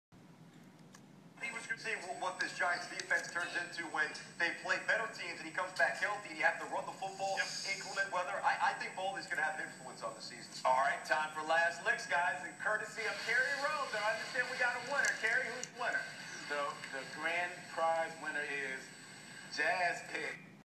Kerry Rhodes announces me as winner of contest for trip to Miami on SNY
I was the winner. Didn't know he was gonna say my name. Fucking nuts . Thanks Kerry. All inclusive trip to Miami. Front row tix, met the whole Jets team and stayed in same hotel.